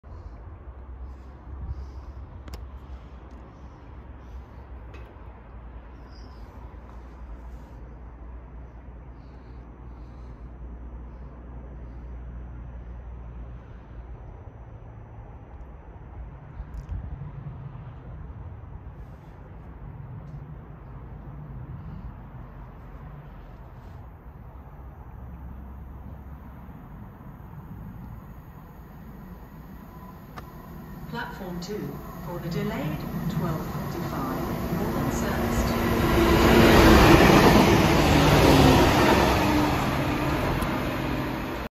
2022-08-27 DR73114 + DR77904
6U12 Sleaford Tamper Siding to High Marnham Powergen
I caught the tripod and stopped the recording early! DOH!